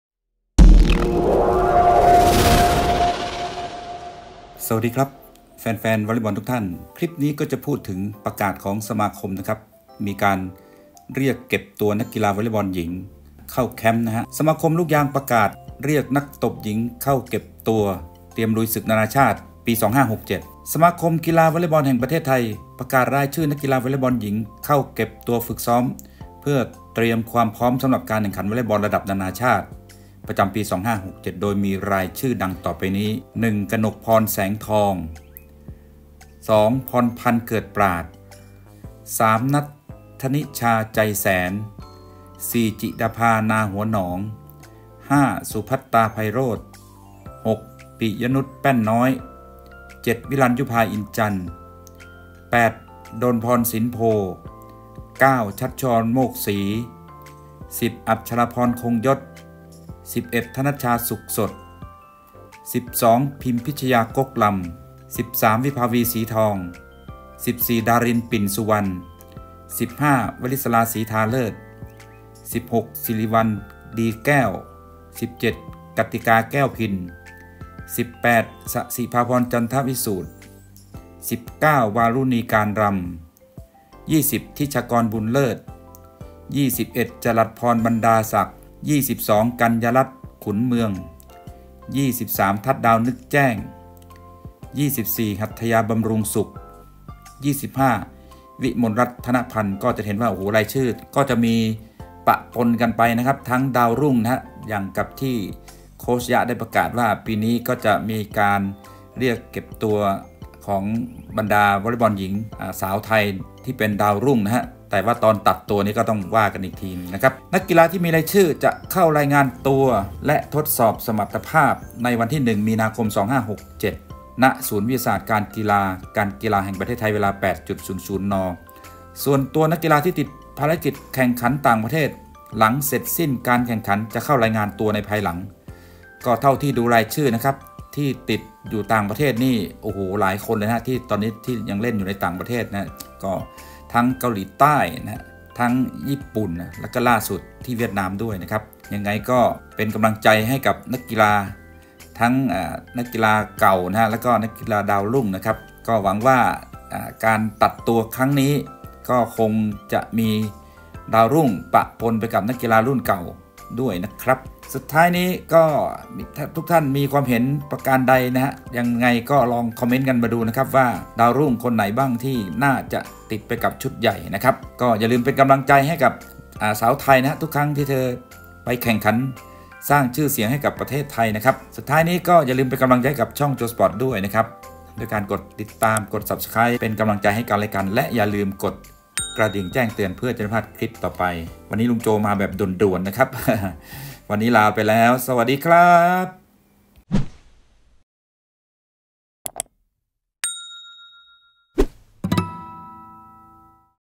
ประกาศรายชื่อ วอลเลย์บอลหญิง เข้าแคมป์ทีมชาติ ลุยศึกนานาชาตฺ 2567,@Jowsports
สมาคมลูกยางประกาศเรียกนักตบหญิงเข้าเก็บตัวเตรียมลุยศึกนานาชาติ 2567
สมาคมกีฬาวอลเลย์บอลแห่งประเทศไทย ประกาศรายชื่อนักกีฬาวอลเลย์บอลหญิงเข้าเก็บตัวฝึกซ้อม เพื่อเตรียมความพร้อมสำหรับการแข่งขันวอลเลย์บอลระดับนานาชาติ ประจำปี 2567 โดยมีรายชื่อดังนี้
กนกพร แสงทอง
พรพรรณ เกิดปราช์ญ
ณัฐณิชา ใจแสน
จิดาภา นาหัวหนอง
สุพัตรา ไพโรจน์
ปิยะนุช แป้นน้อย
วิรัลยุพา อินจันทร์
ดลพร สินโพธ์
ชัชชุอร โมกศรี
อัจฉราพร คงยศ
ธนัชชา สุขสด
พิมพิชยา ก๊กรัมย์
วิภาวี ศรีทอง
ดาริน ปิ่นสุวรรณ
วริศรา สีทาเลิศ
สิริวัลย์ ดีแก้ว
กัตติกา แก้วพิน
ศศิภาพร จันทรวิสูตร
วารุณี การรัมย์
ฑิชากร บุญเลิศ
จรัสพร บรรดาศักดิ์
กัญญารัตน์ ขุนเมือง
ทัดดาว นึกแจ้ง
หัตถยา บำรุงสุข
วิมลรัตน์ ทะนะพันธุ์
นักกีฬาที่มีรายชื่อจะเข้ารายงานตัวและทดสอบสมรรถภาพ ในวันที่ 1 มีนาคม 2567 ณ ศูนย์วิทยาศาสตร์การกีฬา การกีฬาแห่งประเทศไทย เวลา 08.00 น. ส่วนนักกีฬาที่ติดภาระกิจแข่งขันต่างประเทศ หลังเสร็จสิ้นการแข่งขันจะเข้ารายงานตัวในภายหลัง